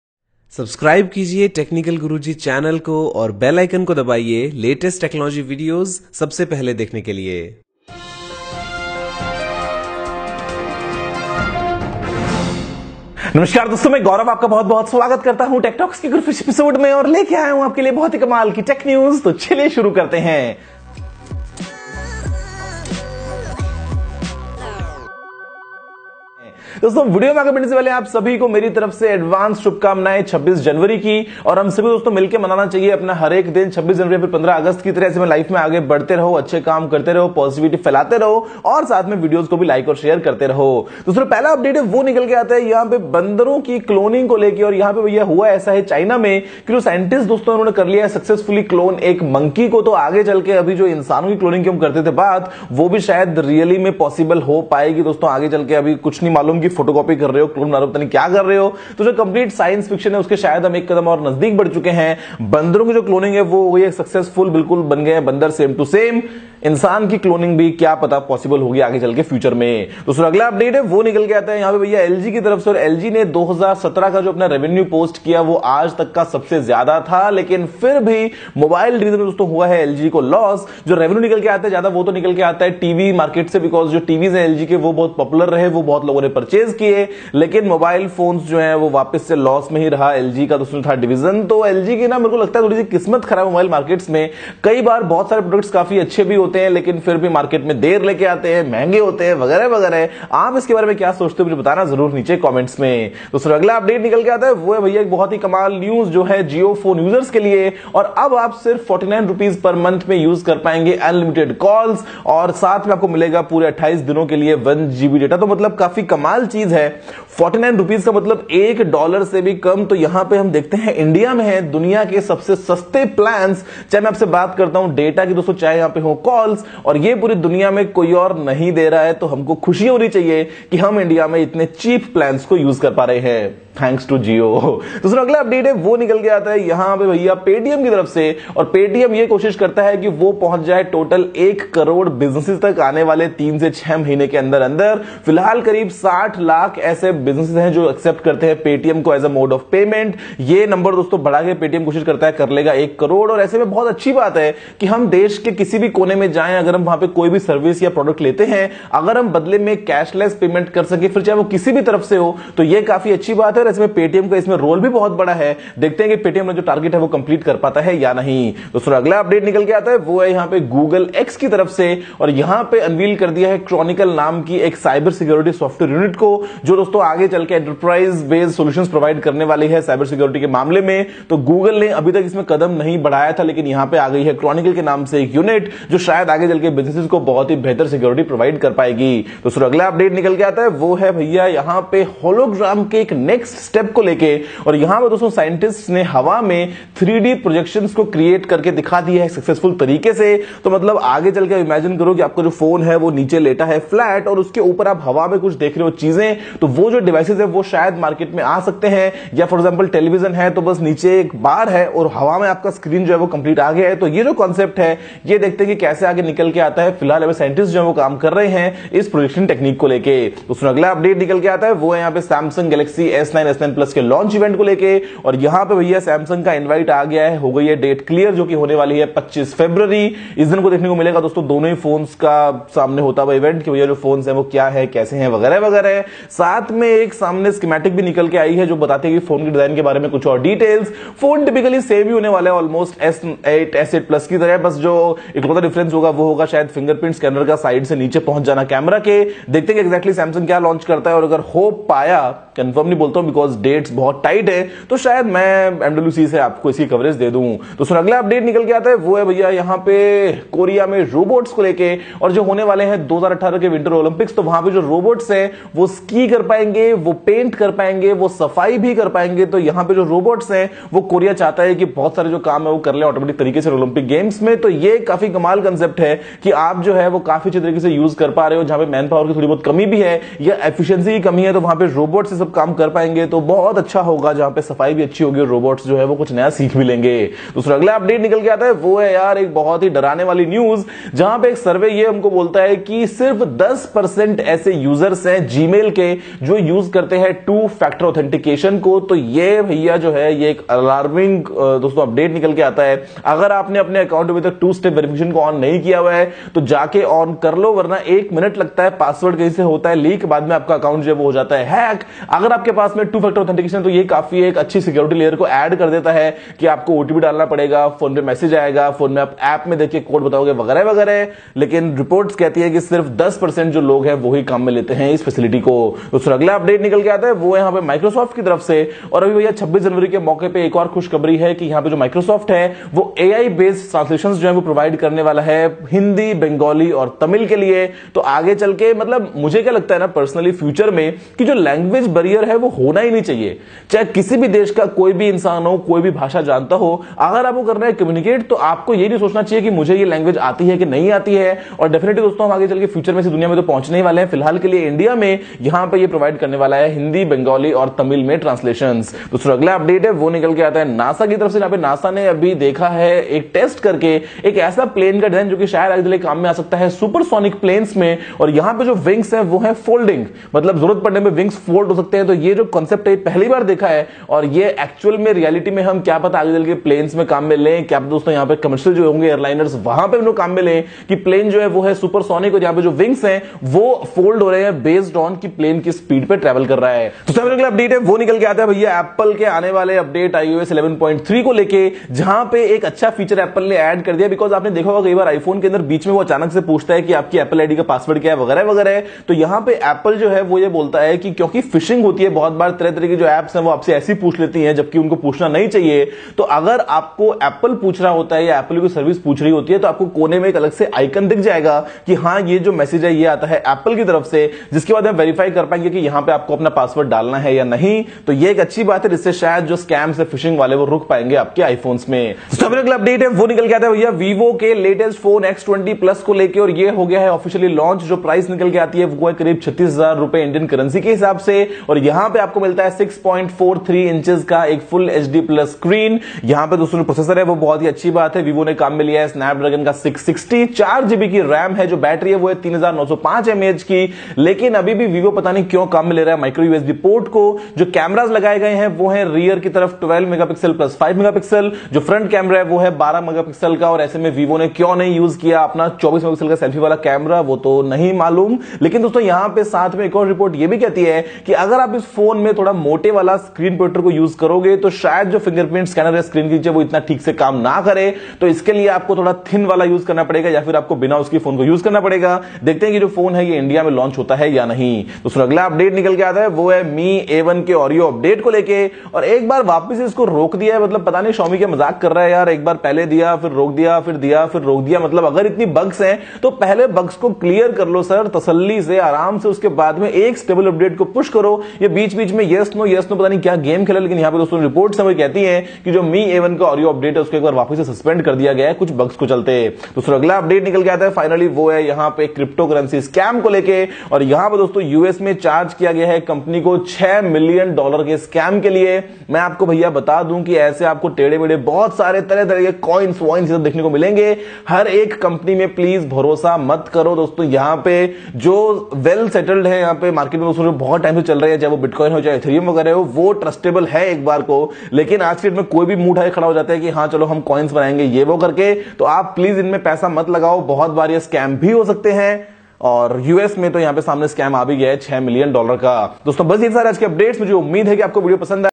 Latest tech talks with Technical Guruji. Jio 49 plans, Paytm Business, Galaxy S9 launched etc
Tik talks